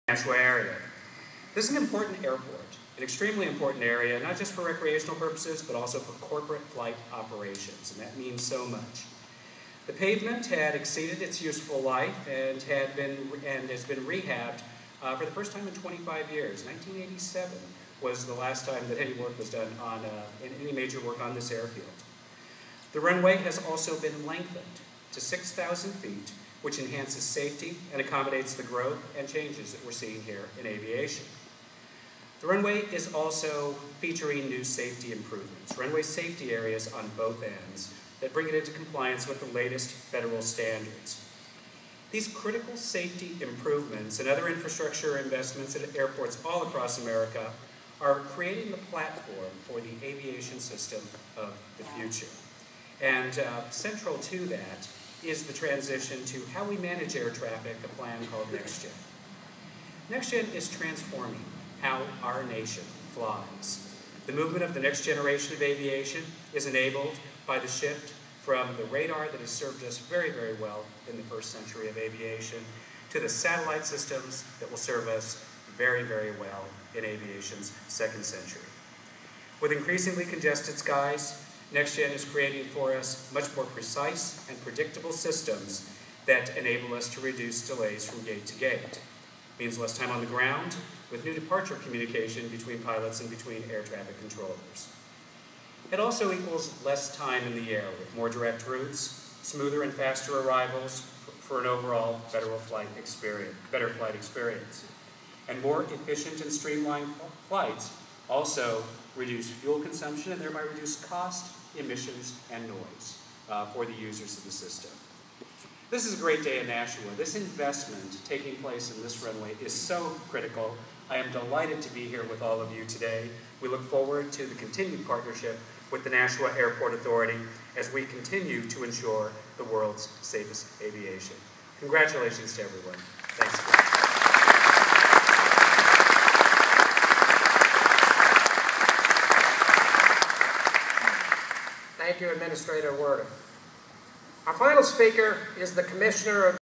Acting FAA Administrator Huerta at Nashua Airport
Attending the ribbon cutting ceremony for the new Runway 14/32